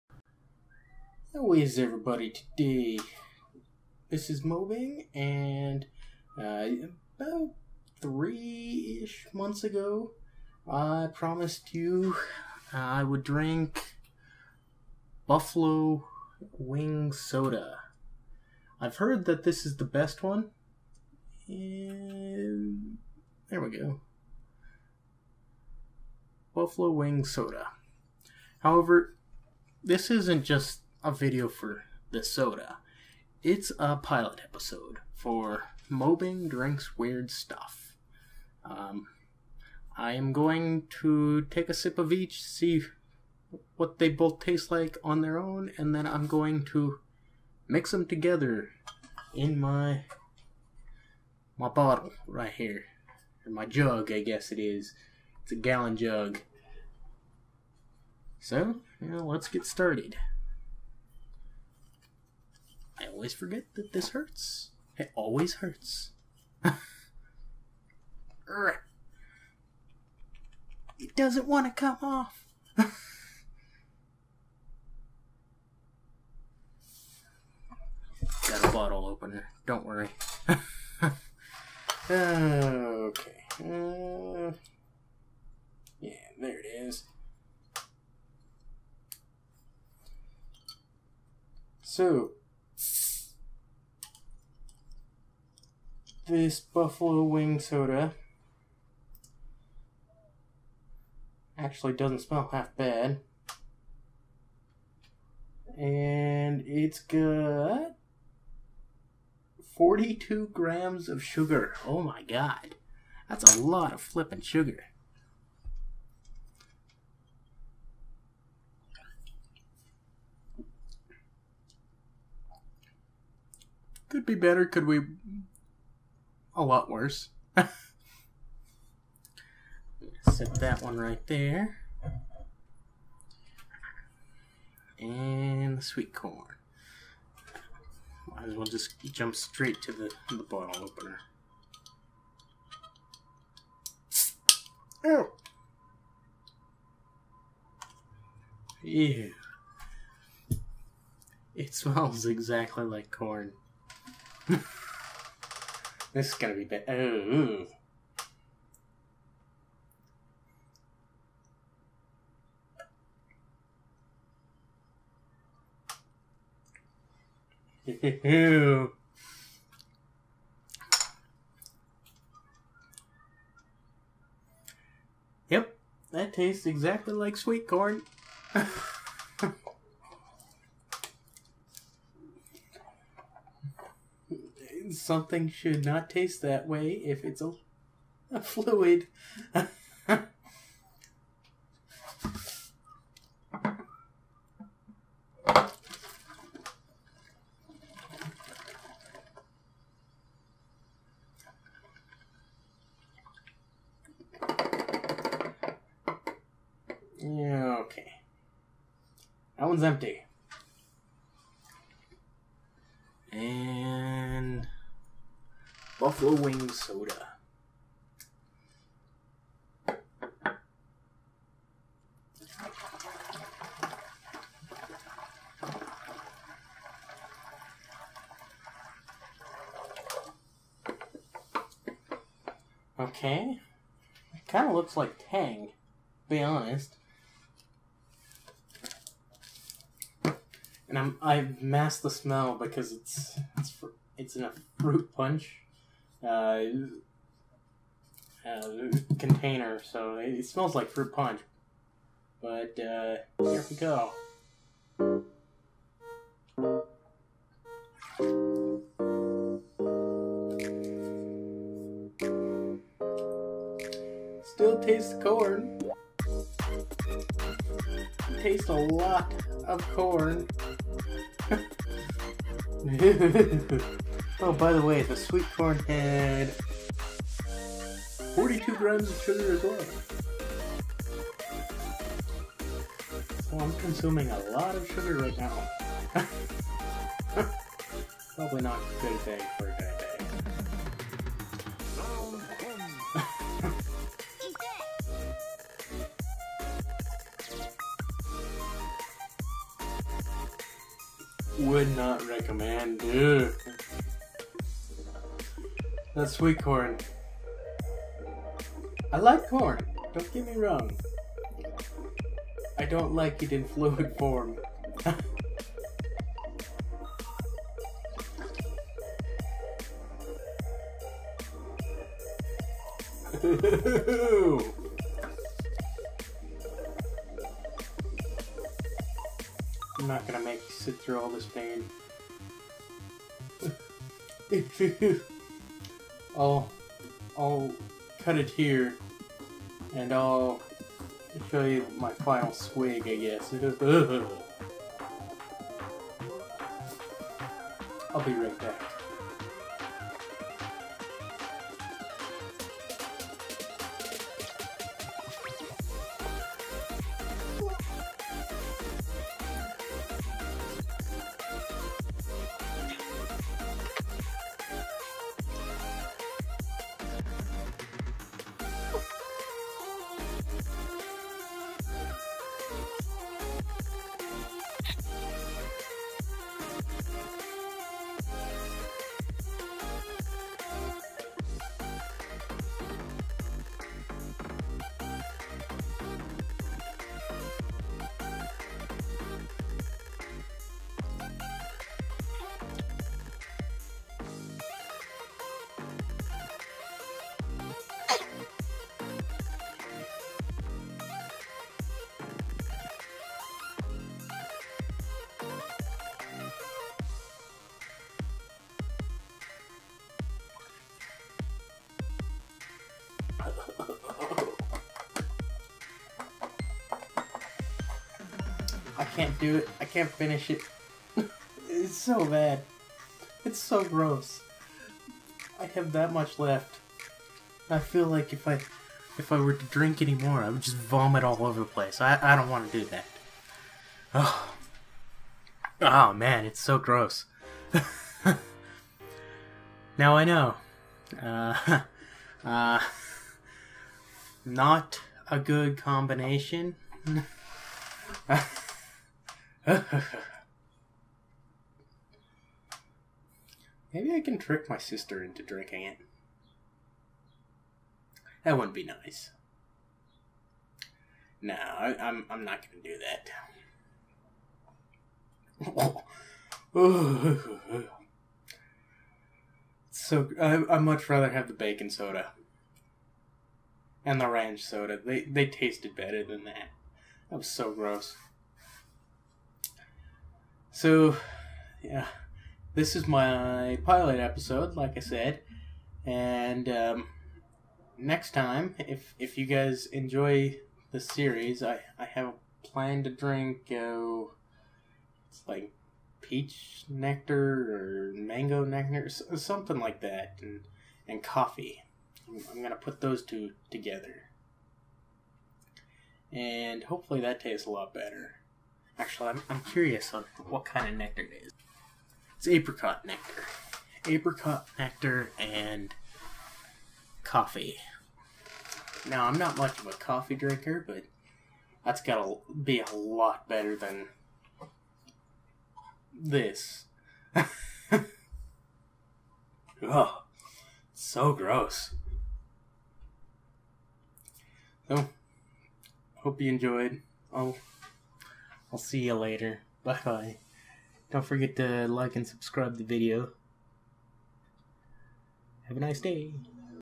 Mobinng Drinks Weird Stuff 1
This is a pilot episode of a series where I , Mobinng drink odd concoctions, in this video I combine a Sweet Corn soda from Lester's Fixin' and a Buffalo Wing soda from the same company. I ask myself why I drank it, because though they might not taste very good separate, they taste even worse together. But that is the entire point to test to see if things go together.